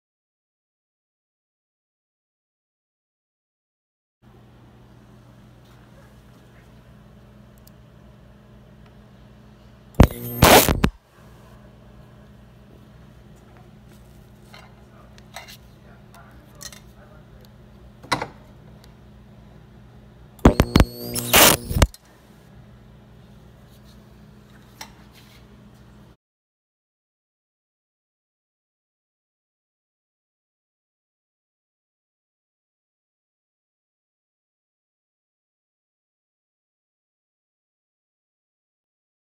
Mineral Insulated wire stripping w/ Ultrasonics (MI Wire Stripper)
Quickly and safely strip mineral insulated wires with Sonitek Ultrasonic Welders.